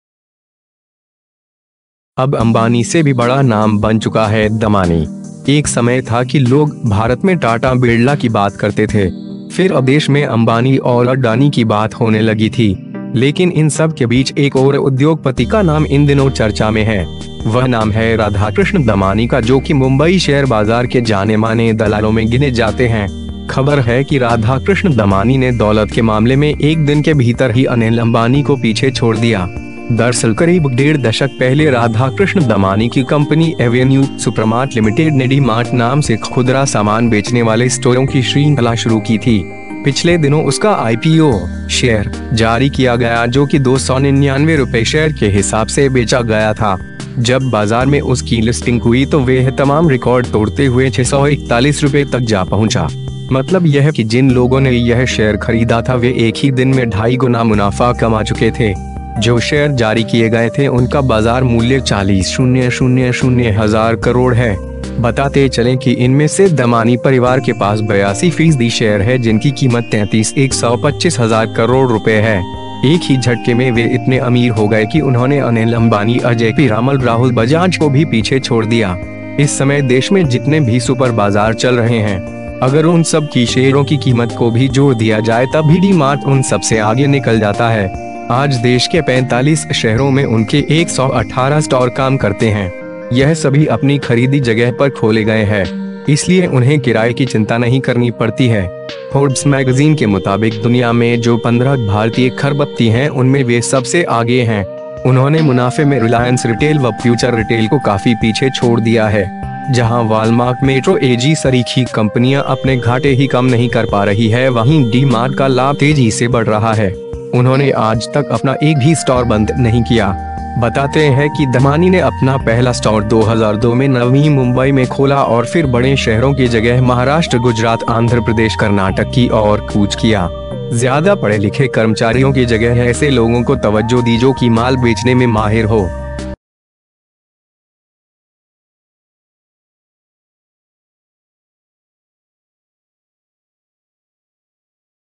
अब अंबानी से भी बड़ा नाम बन चुका है दमानी
एक समय था कि लोग भारत में टाटा, बिड़ला की बात करते थे. फिर अब देश में अंबानी और अडानी की बात होने लगी थी.

लेकिन इन सबके बीच एक ओर उद्योगपति का नाम इन दिनों चर्चा में है. वह नाम है राधाकृष्ण दमानी का, जो कि मुंबई शेयर बाजार के जाने माने दलालों में गिने जाते हैं.

खबर है कि राधाकृष्ण दमानी ने दौलत के मामले में एक दिन के भीतर ही अनिल अंबानी को पीछे छोड़ दिया.

दरअसल, करीब डेढ़ दशक पहले राधाकृष्ण दमानी की कंपनी एवेन्यू सुपरमार्ट लिमिटेड ने ‘डी-मार्ट’ नाम से खुदरा सामान बेचने वाले स्टोरों की श्रृंखला शुरू की थी. पिछले दिनों उसका आईपीओ (शेयर) जारी किया गया जो कि 299 रुपए शेयर के हिसाब से बेचा गया था.

जब बाजार में उसकी लिस्टिंग हुई तो वह तमाम रिकार्ड तोड़ते हुए 641 रुपए तक जा पहुंचा. मतलब यह कि जिन लोगों ने यह शेयर खरीदा था वे एक ही दिन में ढाई गुना मुनाफा कमा चुके थे. जो शेयर जारी किए गए थे उनका बाजार मूल्य 40,000 हजार करोड़ है.

बताते चलें कि इनमें से दमानी परिवार के पास 82 फीसदी शेयर है जिनकी कीमत 33,125 हजार करोड़ रुपए है.

एक ही झटके में वे इतने अमीर हो गए कि उन्होंने अनिल अंबानी, अजय पीरामल, राहुल बजाज को भी पीछे छोड़ दिया.

इस समय देश में जितने भी सुपर बाजार चल रहे हैं. अगर उन सबकी शेयरों की कीमत को भी जोड़ दिया जाए तब भी डी-मार्ट उन सबसे आगे निकल जाता है.

आज देश के 45 शहरों में उनके 118 स्टोर काम करते हैं. यह सभी अपनी खरीदी जगह पर खोले गए है. इसलिए उन्हें किराए की चिंता नहीं करनी पड़ती है.

फोर्ब्स मैगजीन के मुताबिक दुनिया में जो 15 भारतीय खरबपति हैं उनमें वे सबसे आगे हैं. उन्होंने मुनाफे में रिलायंस रिटेल व फ्यूचर रिटेल को काफी पीछे छोड़ दिया है. जहां वालमार्ट, मेट्रो एजी सरीखी कंपनियां अपने घाटे ही कम नहीं कर पा रही है वहीं डी-मार्ट का लाभ तेजी से बढ़ रहा है. उन्होंने आज तक अपना एक भी स्टोर बंद नहीं किया.

बतातें है कि दमानी ने अपना पहला स्टोर 2002 में नवीं मुंबई में खोला और फिर बड़ें शहरों की जगह महाराष्ट्र, गुजरात, आंध्र प्रदेश, कर्नाटक की और कूच किया. ज्यादा पढ़े लिखे कर्मचारियों की जगह ऐसे लोगों को तवज्जो दी जो कि माल बेचने में माहिर हो.